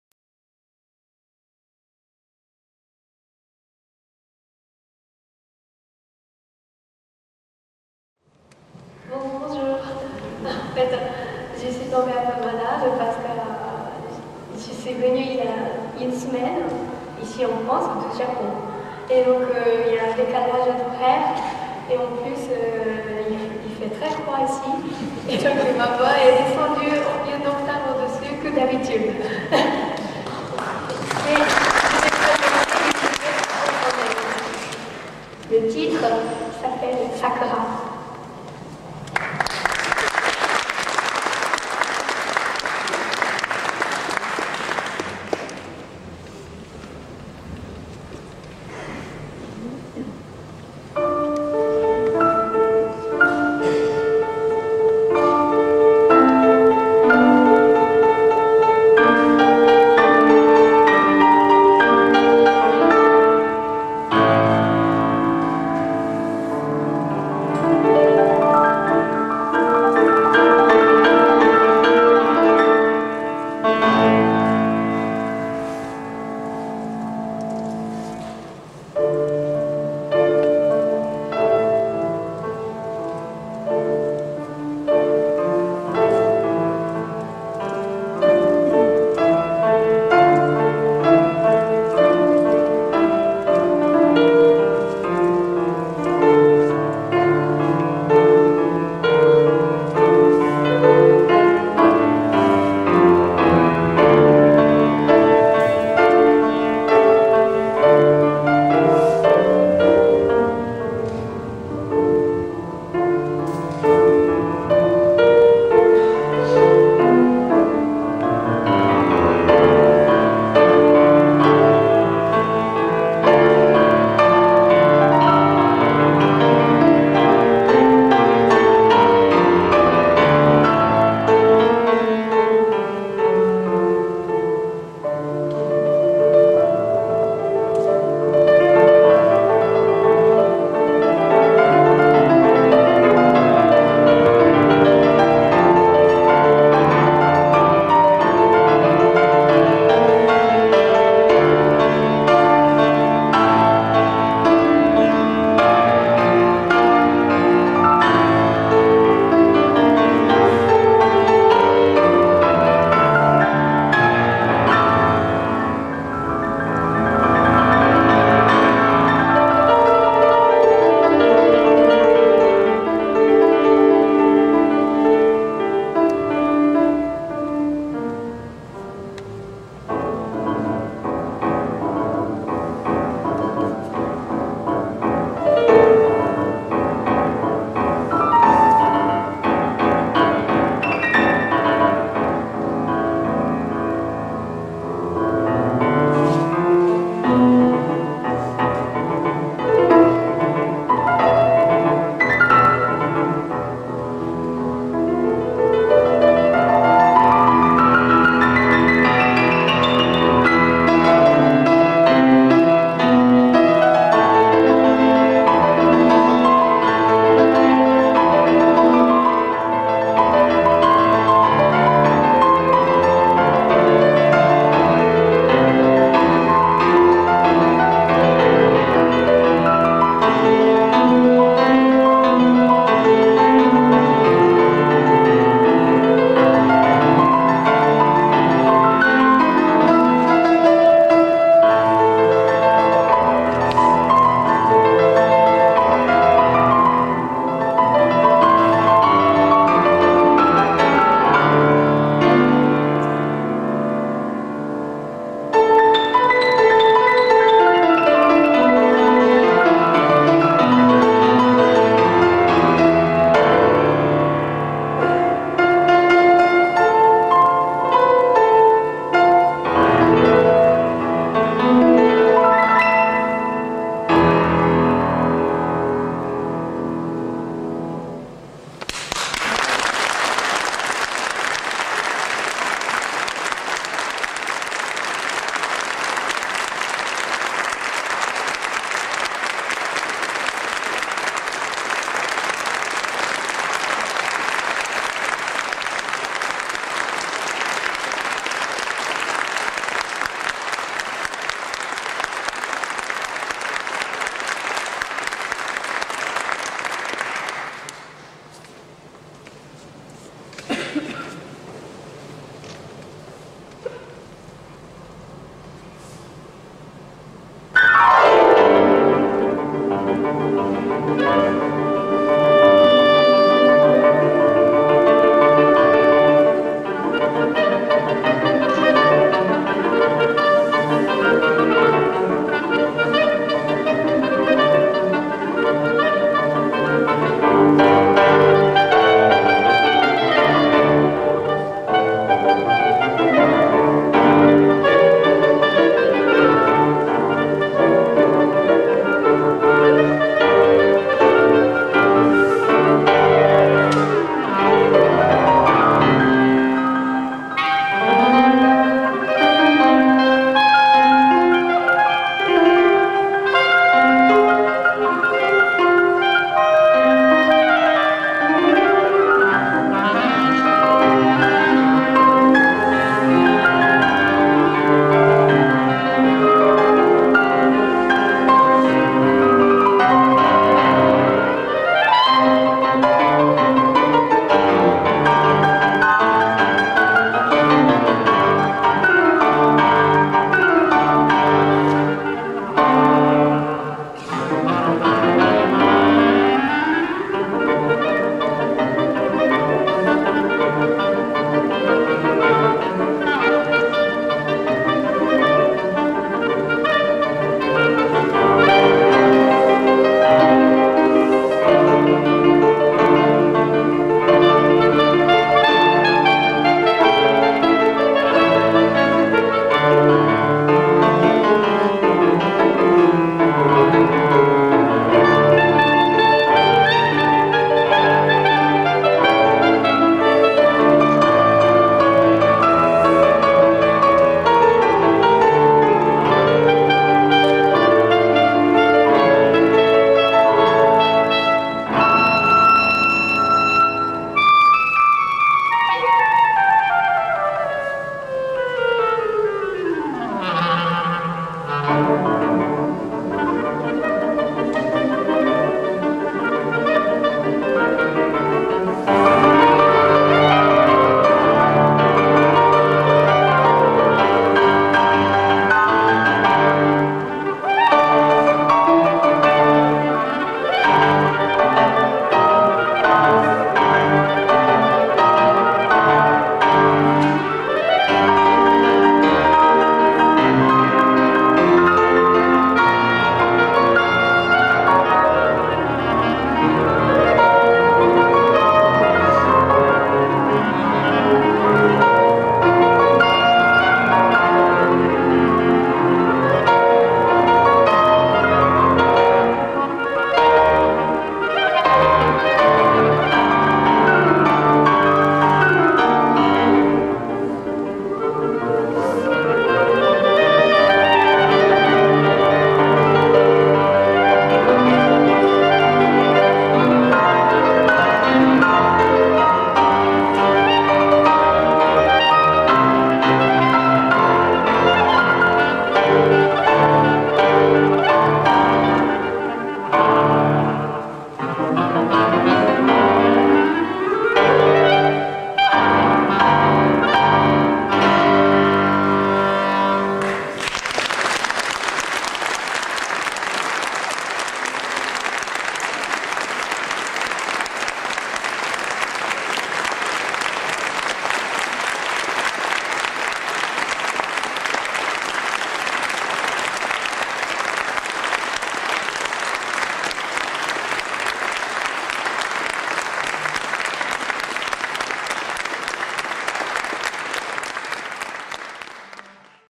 Récital clarinette-piano à Avion
Récital clarinette-piano par Guy Dangain (clarinette) et Shoko Gamo (piano).Concert du 25 Janvier 2009. à Avion 62 (Enregistré par une caméra amateur)